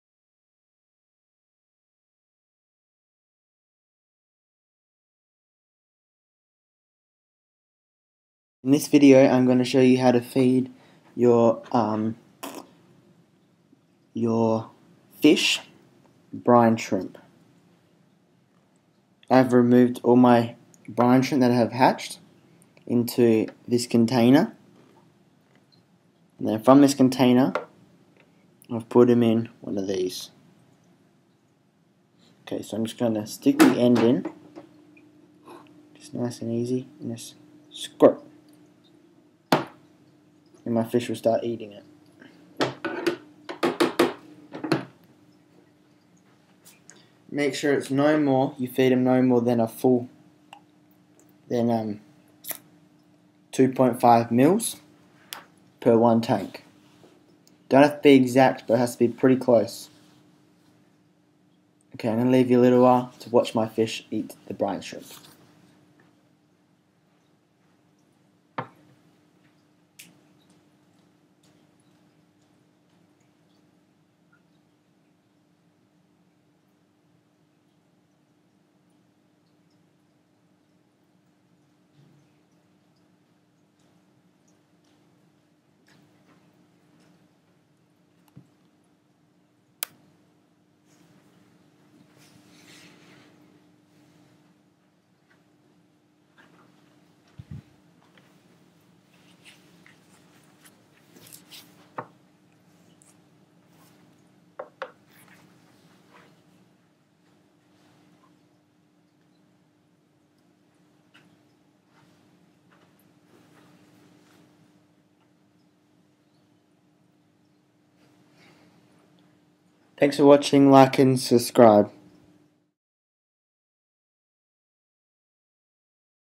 HOW TO feed brine shrimp
Another how to video
this is a video to feed your fish brine shrimp